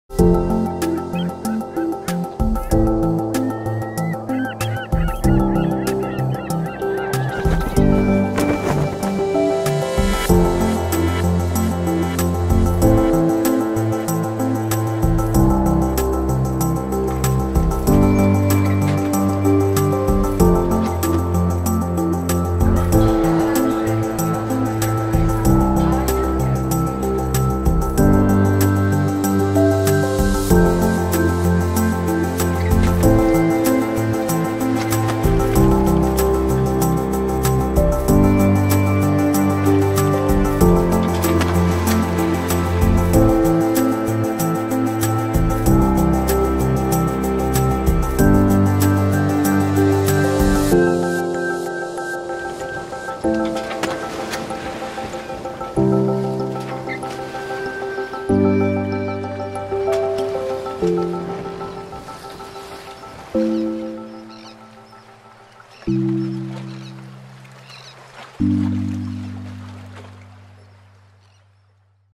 Sailing holidays in Hebrides, St Kilda & Canary Islands aboard Zuza Yacht
Built in 1998, this double hull 72ft yacht is perfect for those who want a bit more comfort, easy sailing and plenty of wildlife spotting. Sailing in the Hebrides around the Small Islands, Mull, Jura, Skye and St Kilda every summer with winter sun holidays in the Canary Islands. No sailing experience needed to join this all female crew for wildlife adventures and foodie experiences.